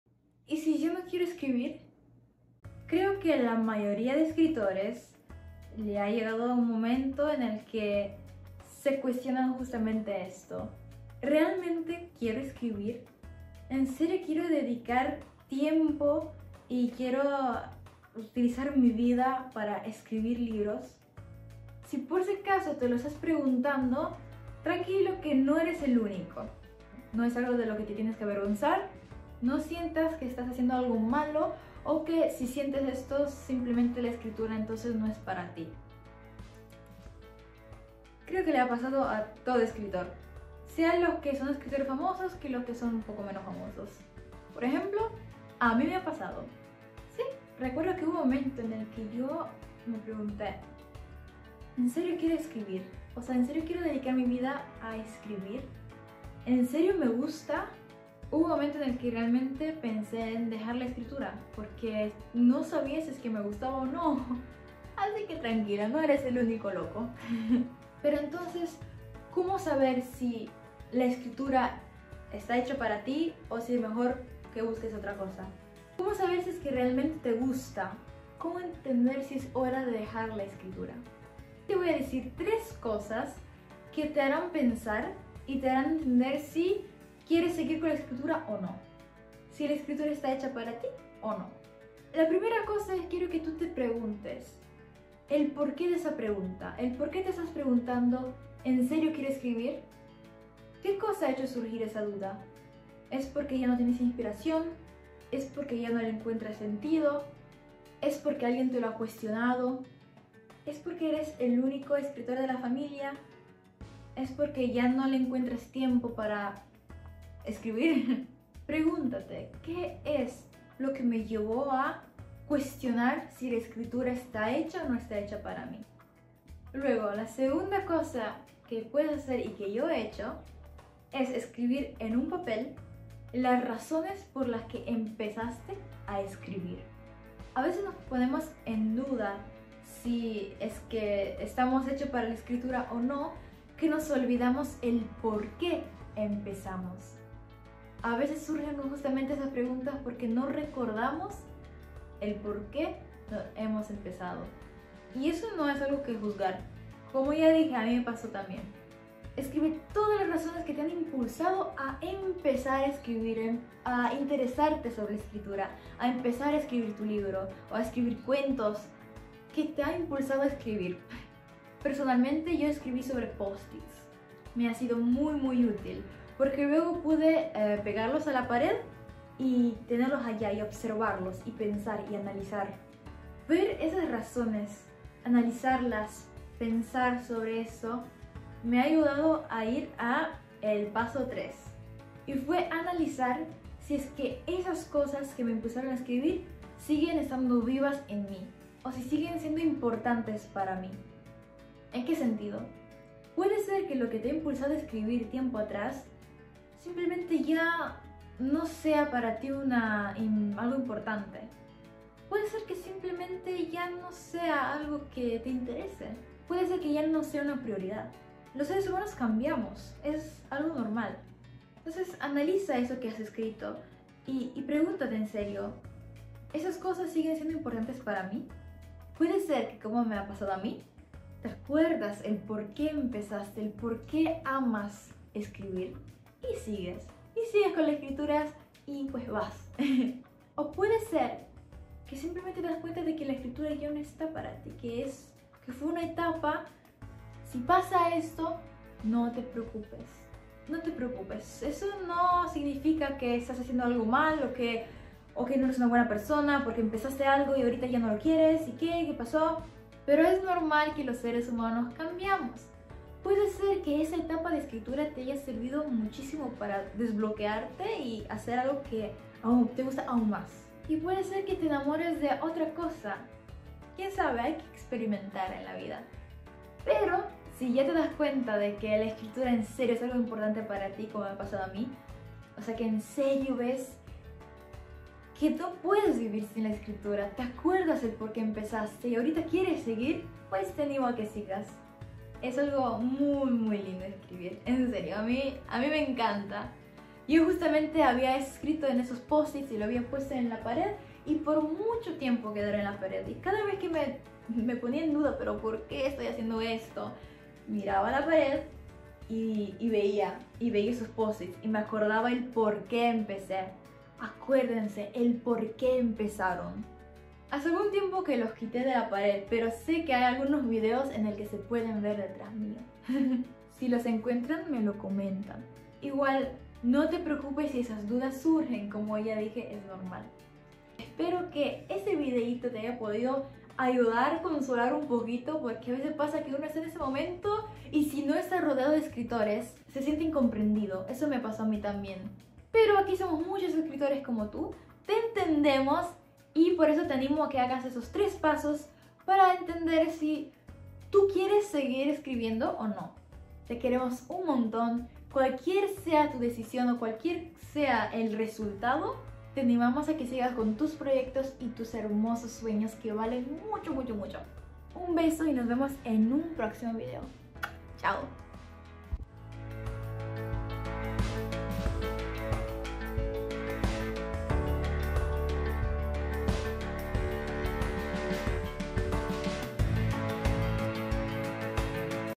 ¿Y si ya no quiero escribir?😯
Todo escritor pasa por un momento en el que se cuestiona si quiere seguir escribiendo o no, si quiere seguir siendo un escritor o no.
Es una duda normal. (A mí me pasó también).
En este video te cuento 3 cosas que puedes hacer para descubrir si, efectivamente, quieres dejar de escribir, o si en realidad, la escritura está hecha para ti.
Espero que este video te ayude de forma practica para resolver esta duda❤️

Si quieren leer los libros que yo he escrito, aquí les dejo el enlace🤗✨